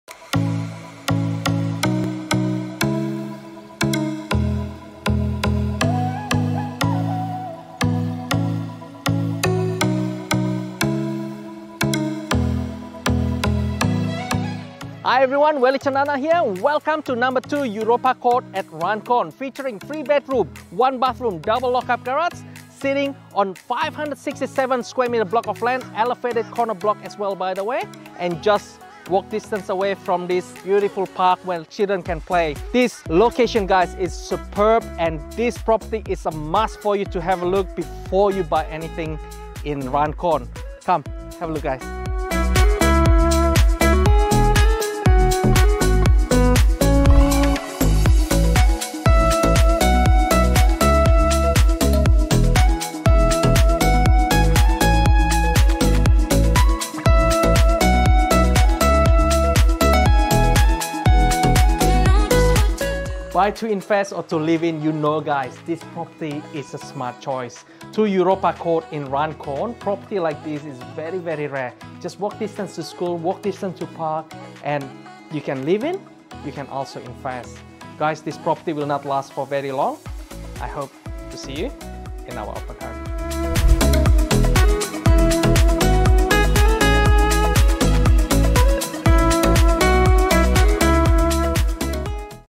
eresidential - Welly Cendana - 2 Europa Court, Runcorn QLD 4113
Charming open plan home in much sought after leafy parkland location!
This solid brick home situate on a raised corner block boasts a spacious 2.5 garage along with large undercover entertainment patio. Virtually ringed by beautiful parklands yet so close to transport and prestigious schools, this lovingly maintained property presents as the ideal purchase for owner-occupiers and investors alike.

Features include:
Air conditioned open plan living and dining area
Modern partly teal coloured Kitchen with Technika electric cooktop and oven.
Ample kitchen cabinet storage and bench space
3 well-sized carpeted bedrooms with BIRs, the master having a ceiling fan
Freshly renovated main bathroom with shower and full length bath
Separate toilet and  laundry rooms
Alfresco entertainment area/patio
Spacious 2.5 garage with separate access
Large garden shed
Insect and security screens throughout
Well established front garden shrubs/trees with very little maintenance required
Built on raised corner block having views of parkland, with the backyard being fully fenced
Within catchment of prestigious Runcorn schools, easy access to public transport and motorways.